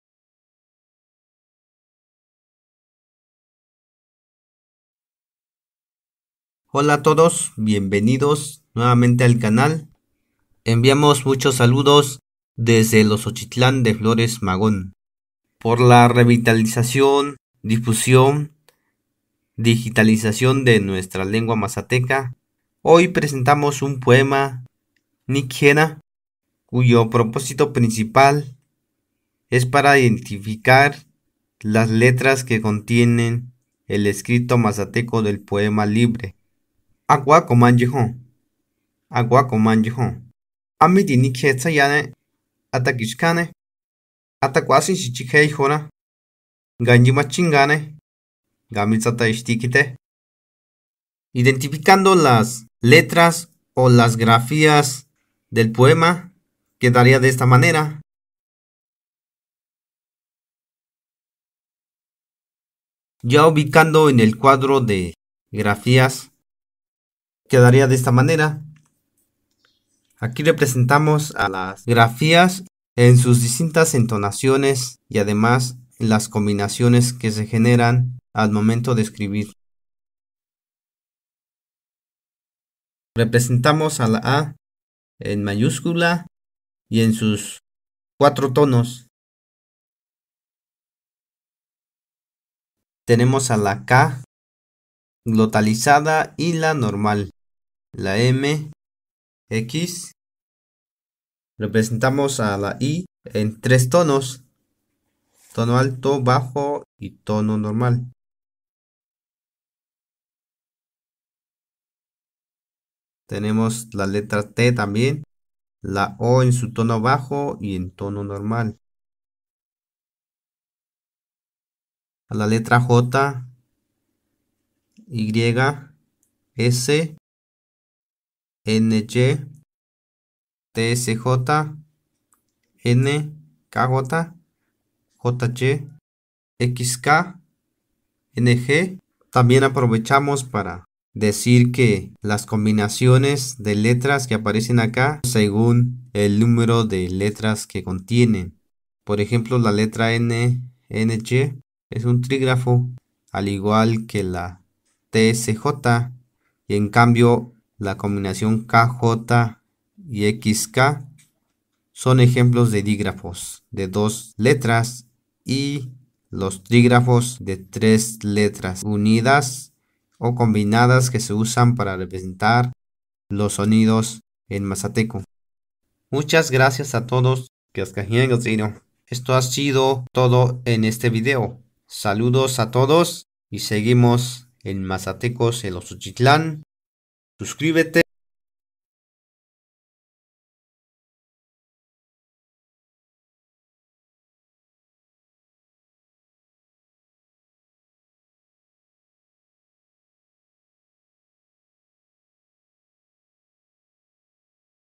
Aprendiendo a escribir y pronunciar mazateco mediante poema
DIGITALIZANDO LA LENGUA MAZATECA DESDE MAZATECO ELOXOCHITLÁN,OAX.